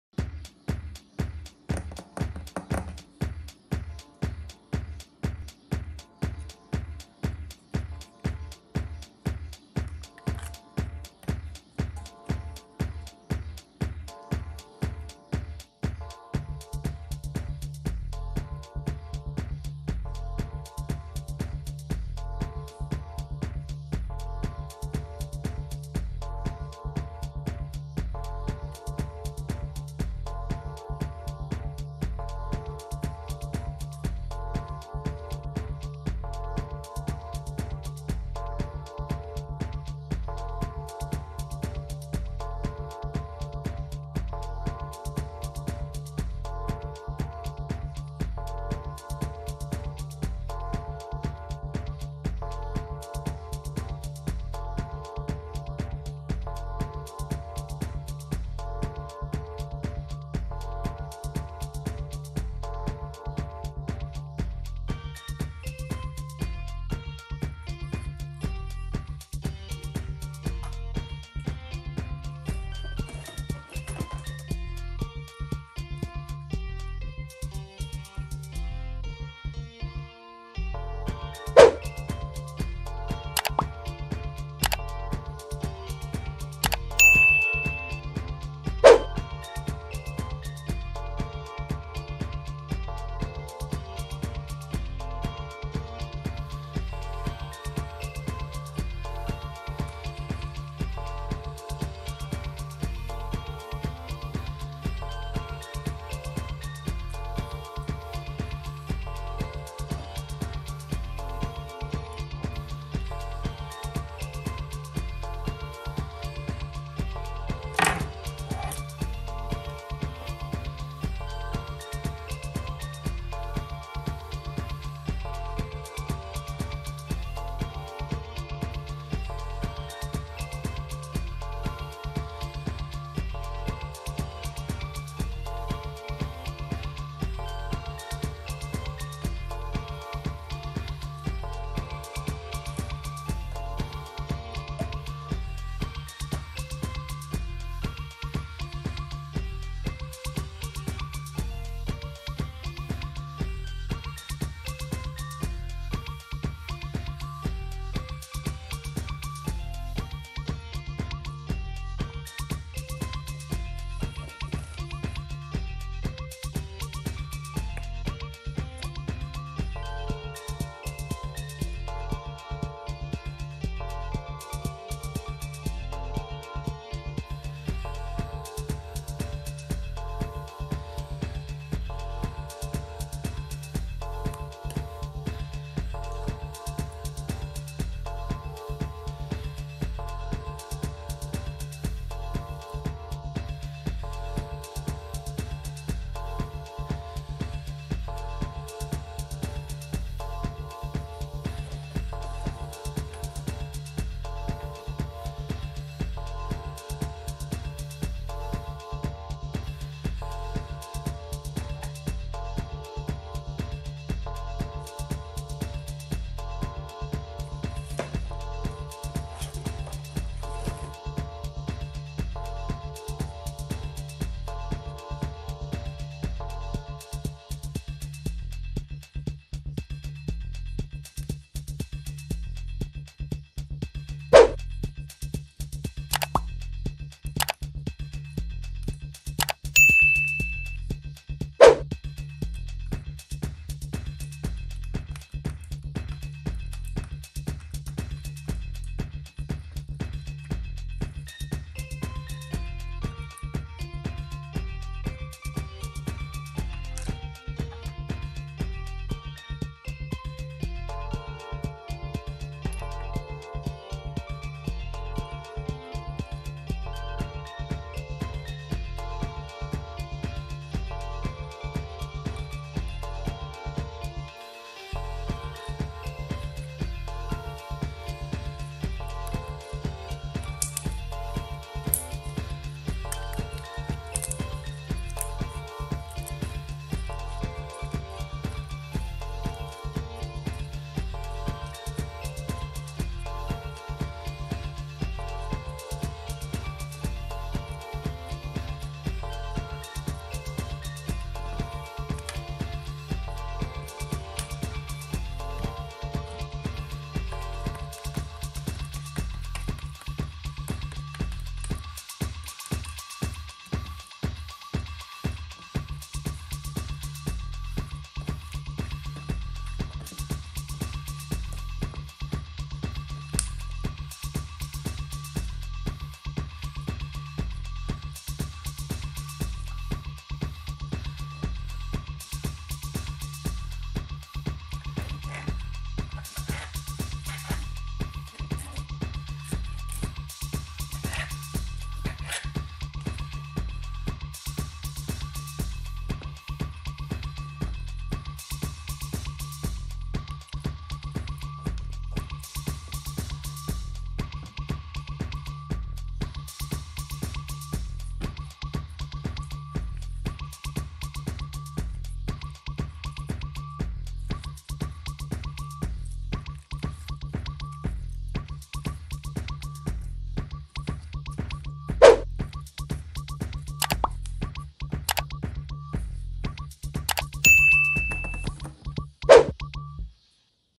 Satisfying slime video | Slime ASMR
Today, I am having fun with clear slime, some colorful and vibrant paints and a lots of glitter. Hope, you also enjoy this idea